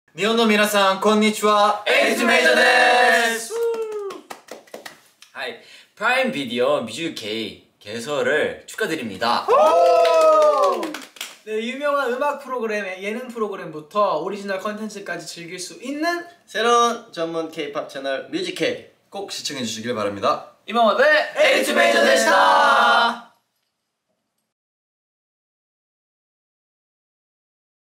82MAJORからお祝いコメント到着！【Music Kオープン記念】
中毒性のある曲と優れたラップの実力が話題！
82MAJOR からスペシャルコメントが到着✨

11月13日(水)Prime Videoに新たにオープンした
K-POP専門チャンネル“Music K”！

近日、彼らの出演番組も…？